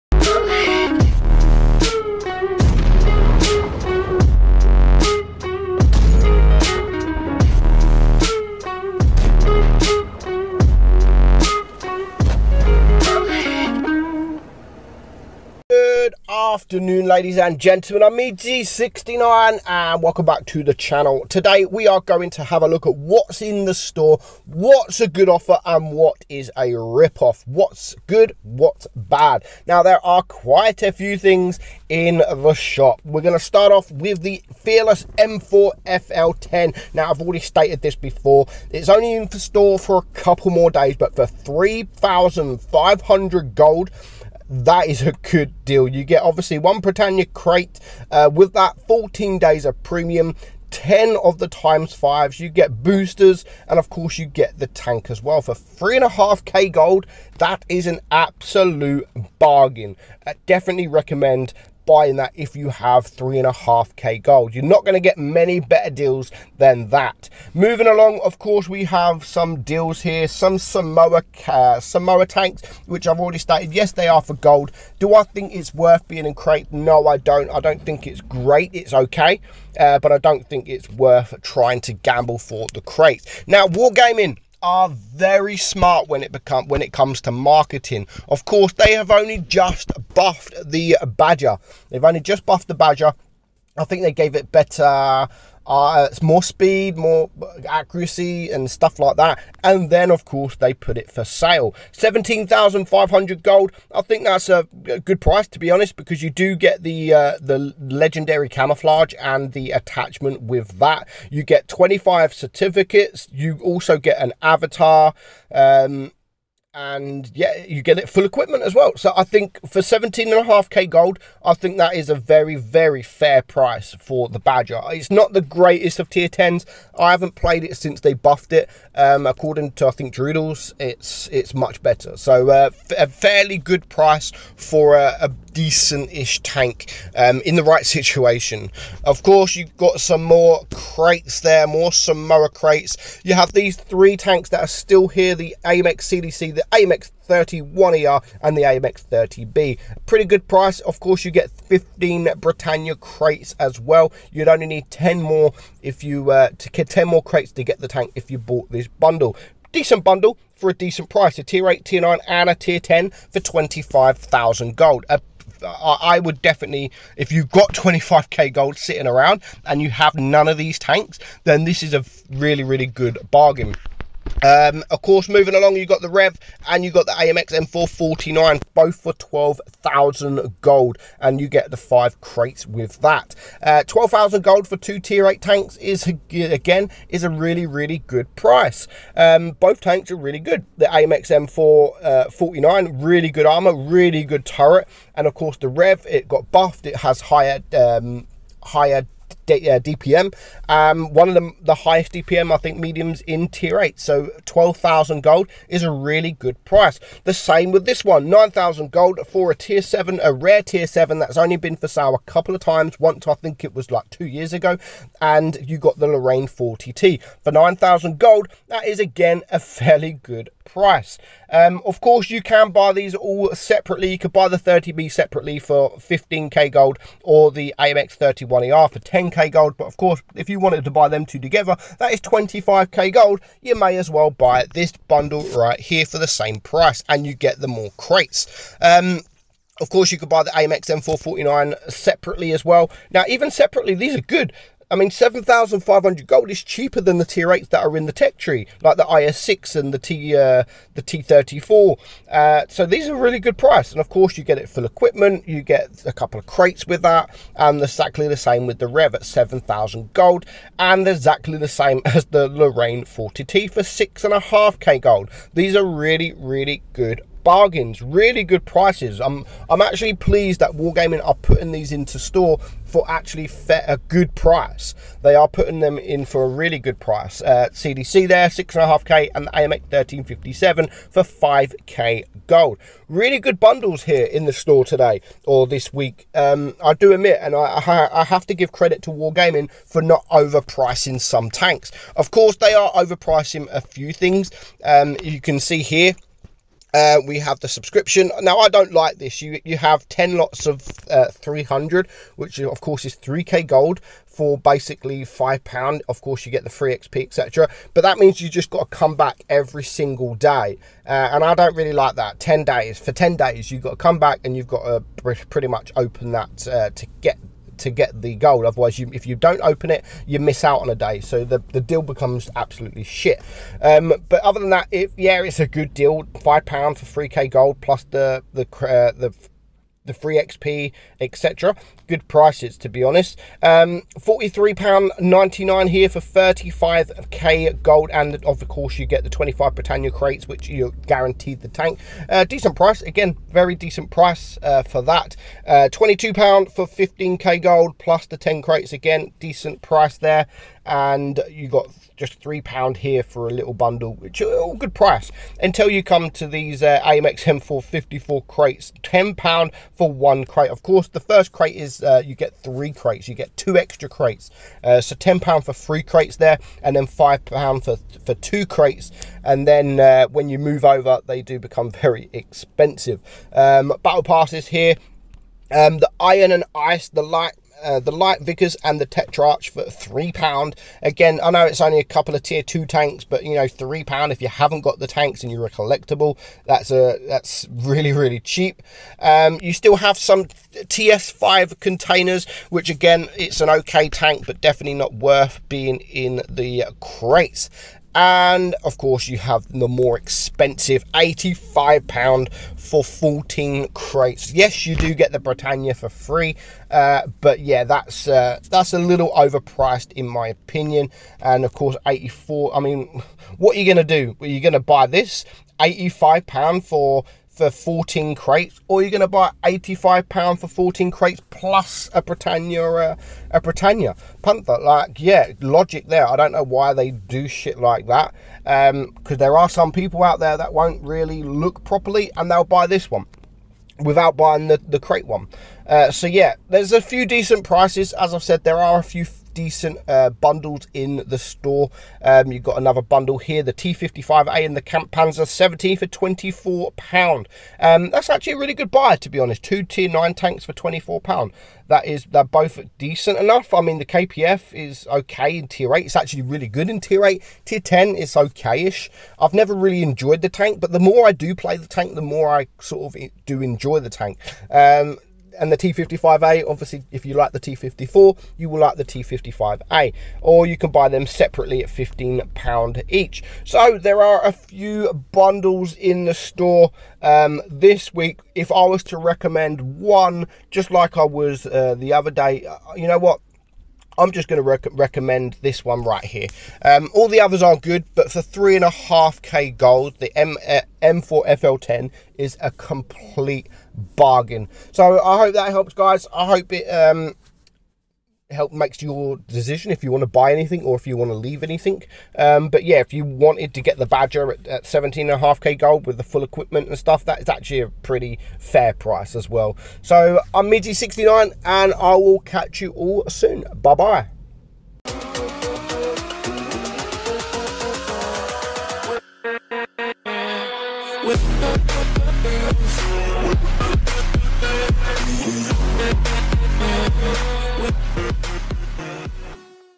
good deals and rip offs.
today we look at what is in the store, what's a good price, and what's a rip off. most things in the store today are actually a really good price, which is nice to see.